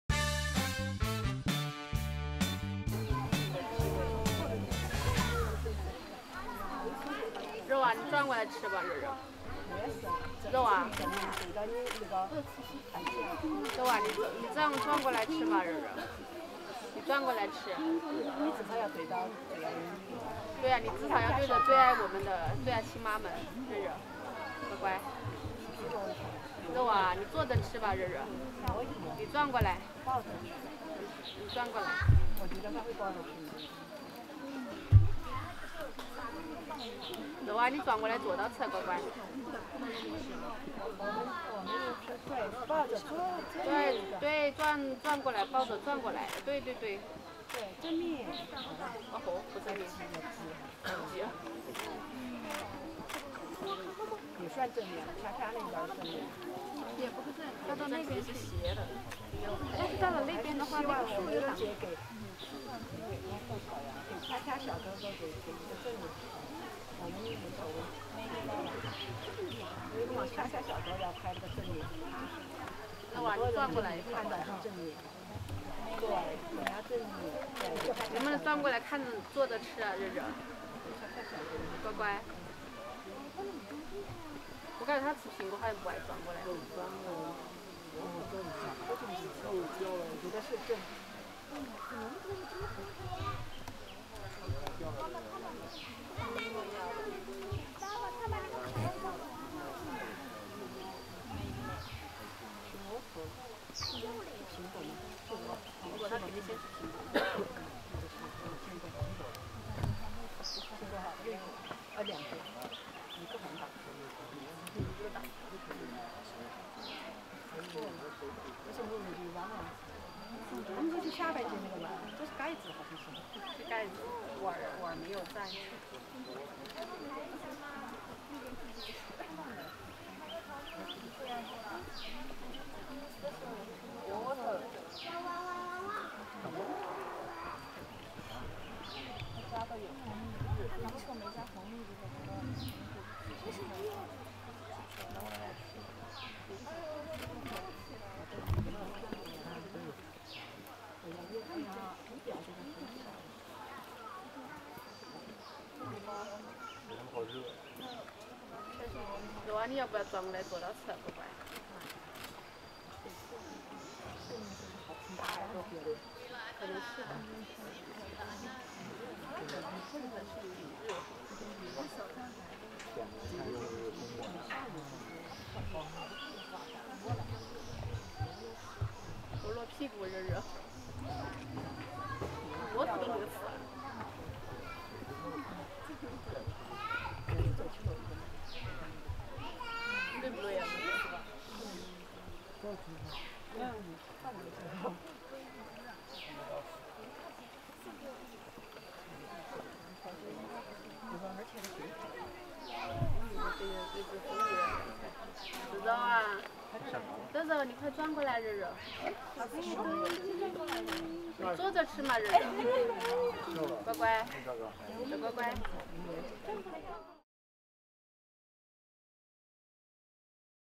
20240404 Broadcast Top Highlights 06
Mei Lan 梅兰（Born in 2016）

Location: No.1 Giant Panda House of Dujiangyan Panda Valley
拍摄地点：都江堰熊猫谷 一号别墅